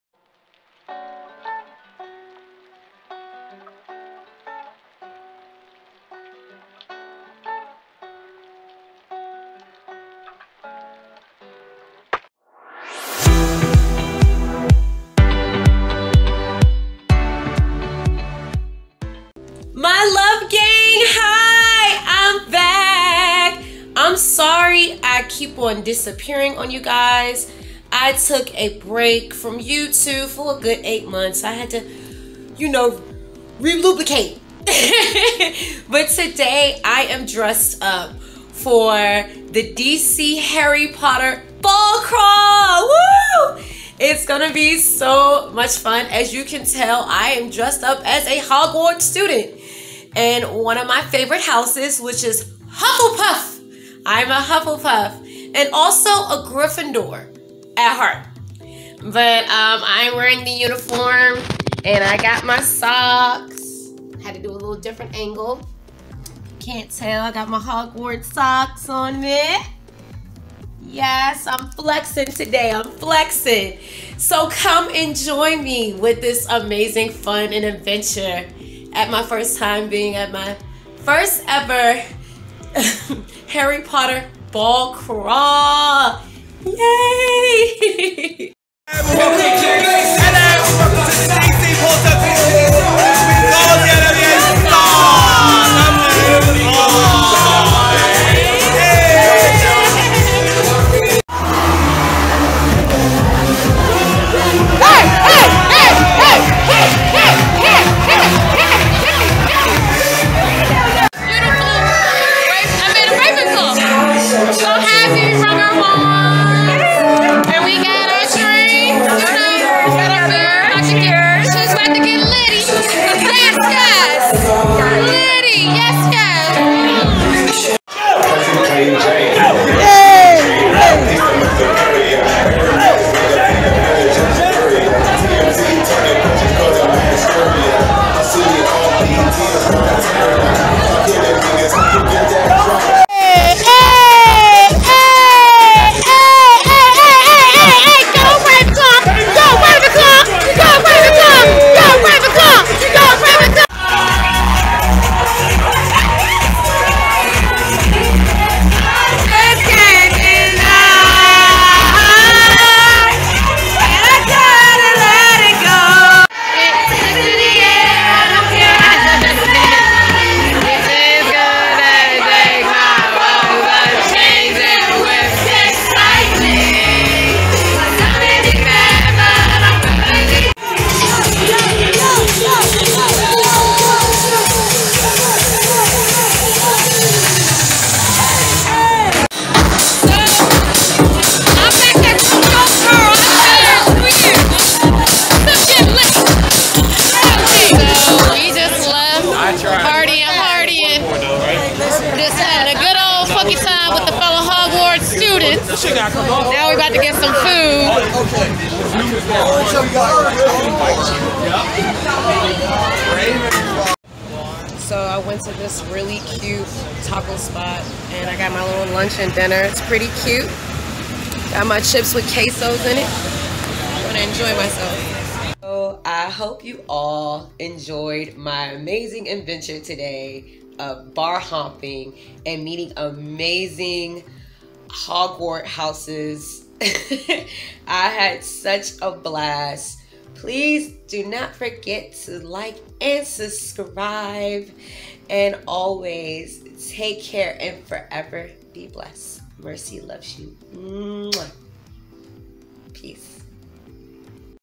At My Very First Harry Potter Bar Crawl ✨🔮 ( Party Vlog)
Hiya! My Love Gang! Long time! No See!!

I miss you all! 💜

Back in September I got to go to my very first Harry Potter Bar Crawl for the first time and I had tons of fun! 💫

I've been a big Harry Potter fan as a kid and I was excited to go to my very first adult bar crawl

A dressed up as one of my favorite Hogwarts houses as a Hufflepuff 💛🖤

( but I'm actually a Gryffindor at heart ❤💛)

but there was a lot of bars, hopping, adult drinks, dancing, twerking, making a lot of fun memories with the other fellow Hogwarts students! 😄🍻🍹

I'm a little embarrassed I got super litty and had such a funky good time you going to catch me twerking in this video 😂😂

it also me being a foodie I had to do a little cheat day with some tacos and nachos! Yum

I will definitely go again real soon! 🔮

I usually put the ending credits but it didn't fit on my video but I'll definitely put it in the next video soon 💛

Mercy Loves You! 💜

Take care and stay forever blessed! 💜

( P.S. just letting you know this video is PG-13 no children under 10 shouldn't be watching this video 📼)